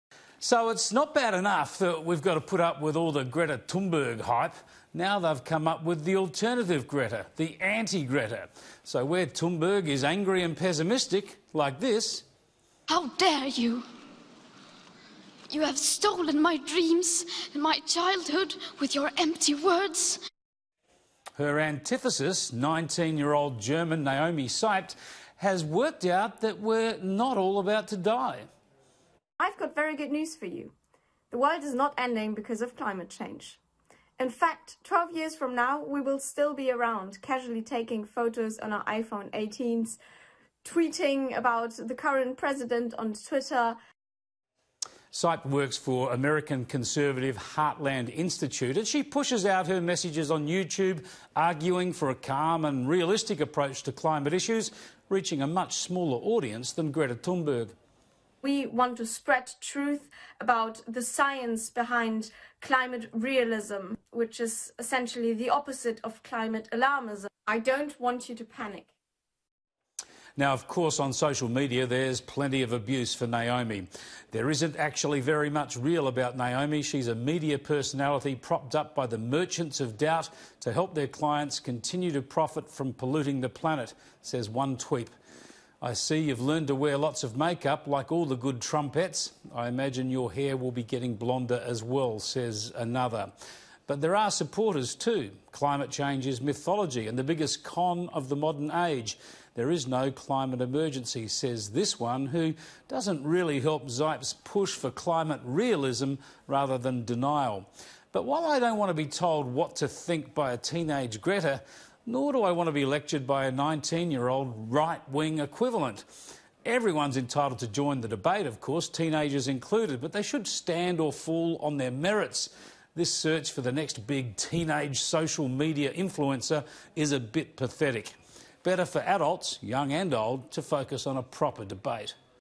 Greta Thunberg’s ‘antithesis’ emerges in the form of a German teenager
Sky News host Chris Kenny says he does not “want to be told what to think by a teenage Greta Thunberg, nor do I want to be lectured by a 19-year-old right-wing equivalent” on climate issues.

Mr Kenny said German teenage conservative social media personality, Naomi Seibt, has emerged as the “antithesis” to Swedish environmentalist Greta Thunberg.

He said Ms Seibt - who is arguing for a “calm and realist approach to climate issues” - "has worked out that we are not all about to die".

“So, it's not bad enough that we have to put up with all the Greta Thunberg hype, now they've come up with the alternative Greta, the anti-Greta," he said.

“This search for the next big teenage social media influencer is a bit pathetic … better for adults, young and old, to focus on a proper debate."

Image: AP